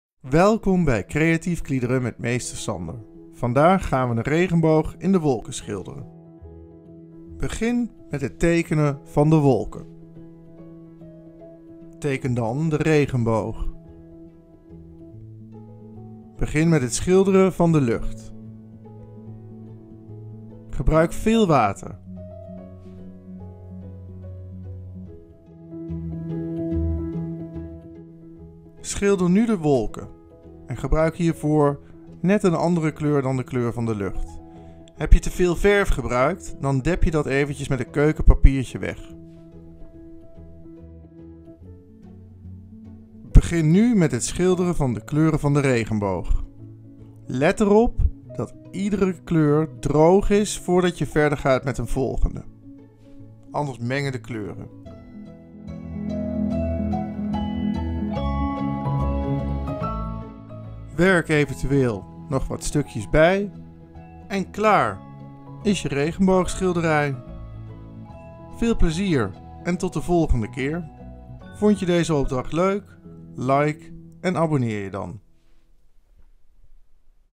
Creatief Kliederen: Regenboog in de wolken - schilderen met aquarelverf
In deze opdracht leert meester Sander je om met aquarelverf te schilderen met een voorbeeld van een regenboog in de wolken. Veel plezier!